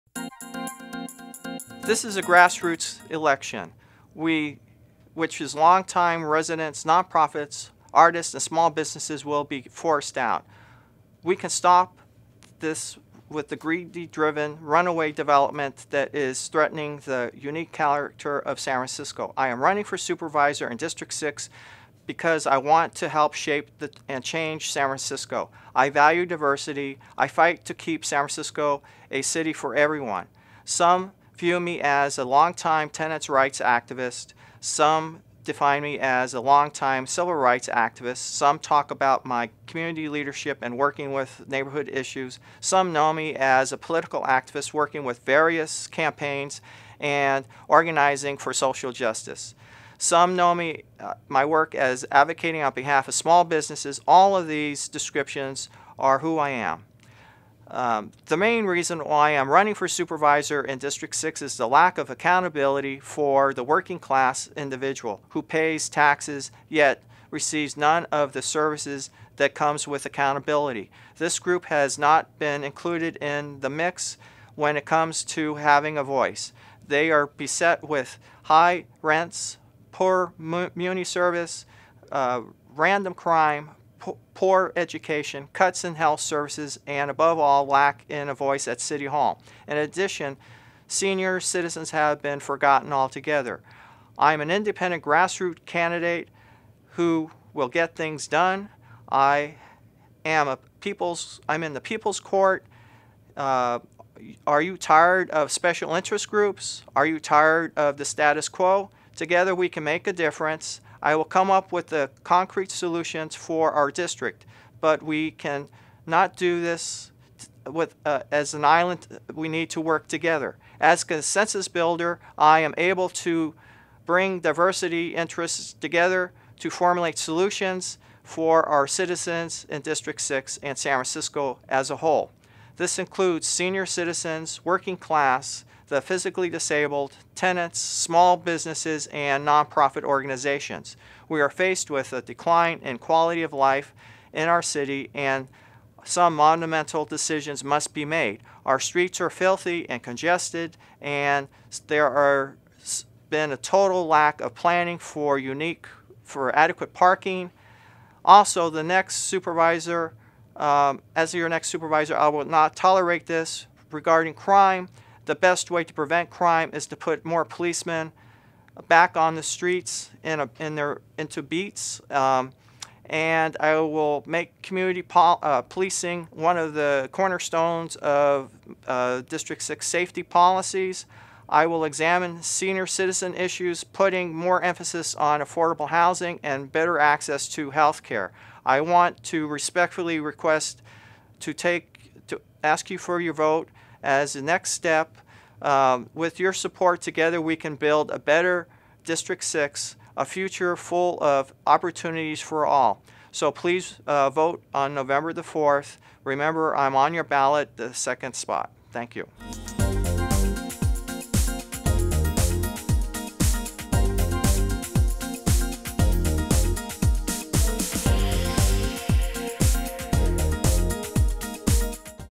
Michael Nulty, Candidate for Supervisor, District 6
Michael Nulty, Candidate for Supervisor, District 6, speaks about his candidacy, and important issues facing San Francisco.